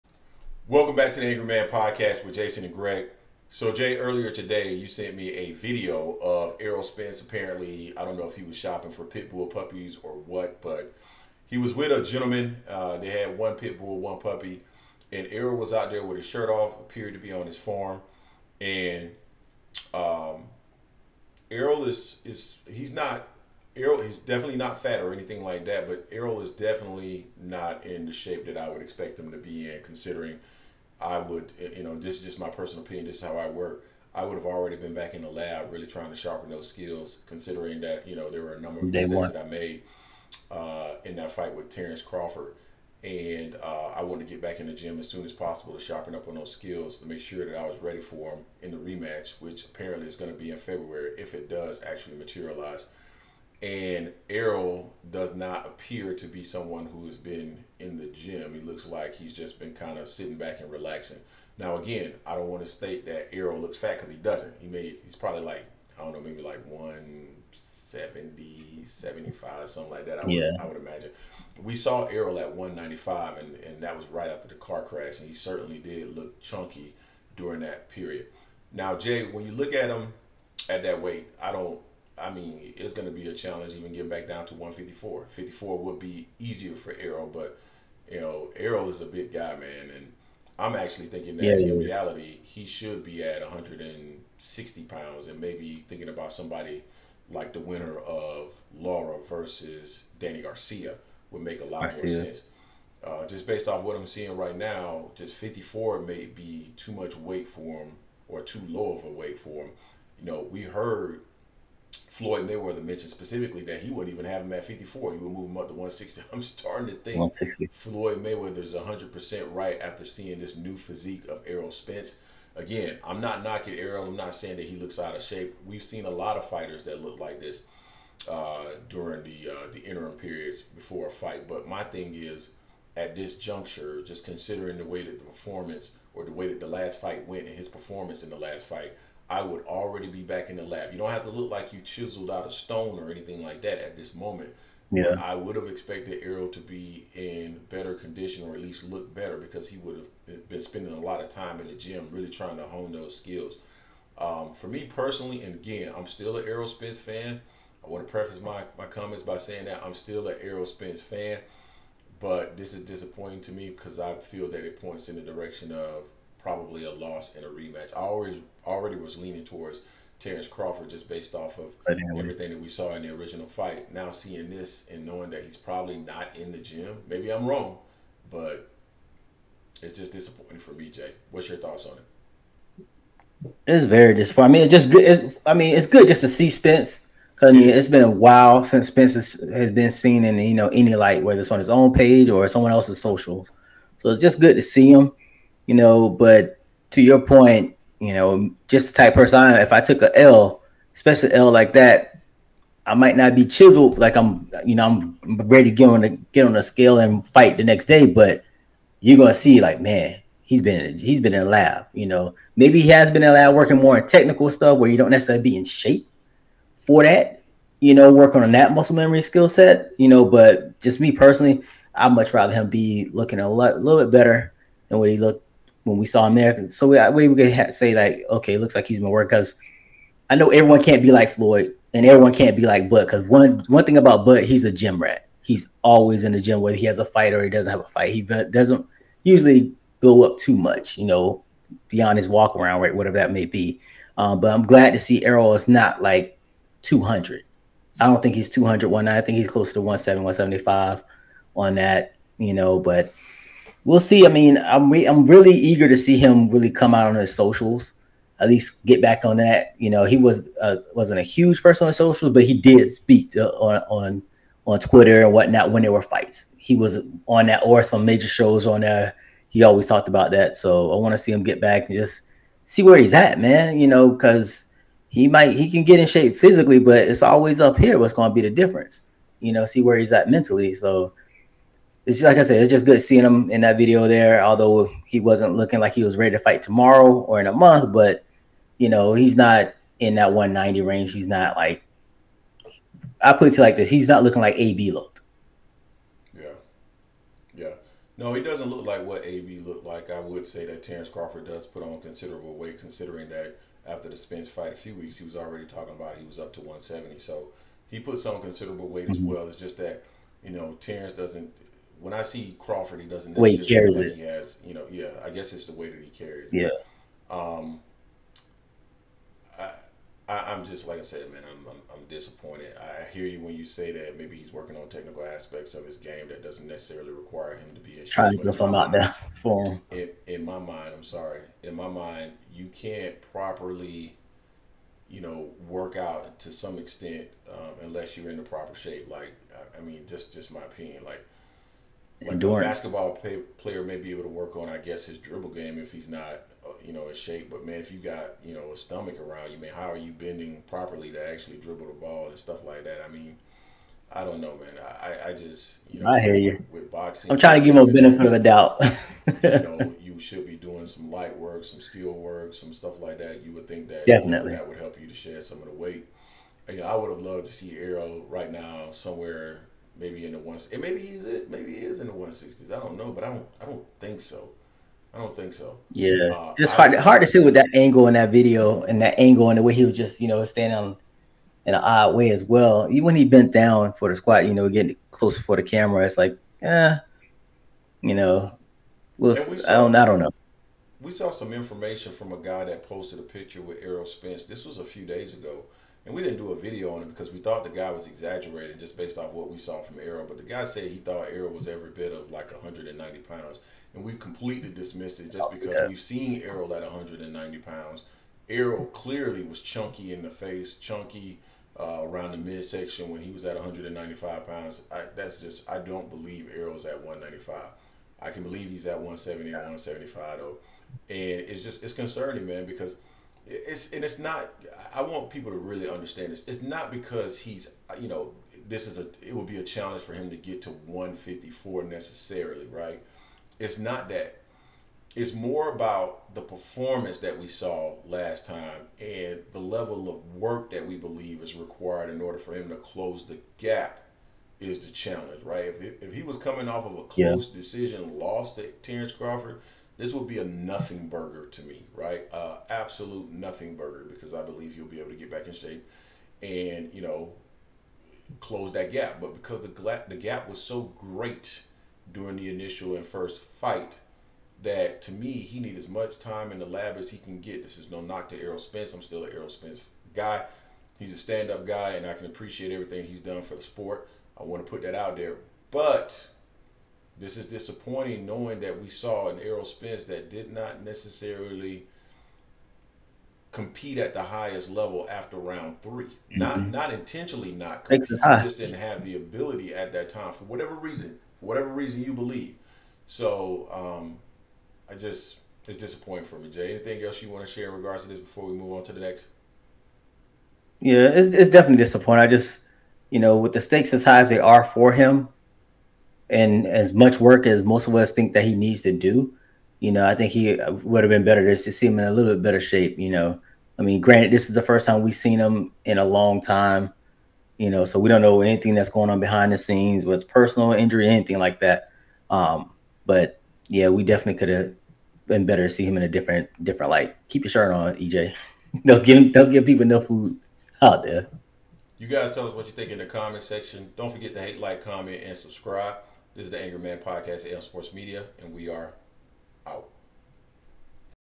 AFTER LATEST ERROL SPENCE SIGHTING, SPENCE SHOULD FORGET TERENCE CRAWFORD REMATCH, MOVE TO 160!!!
Errol Spence has triggered the rematch clause against Terence Crawford. While Spence initially expressed his desire for a rematch at 154 pounds, a recent Twitter post from him suggests that he might want to contemplate moving up to 160 pounds and potentially abandoning the rematch with Crawford. In this video, we delve into the reasons behind this intriguing possibility.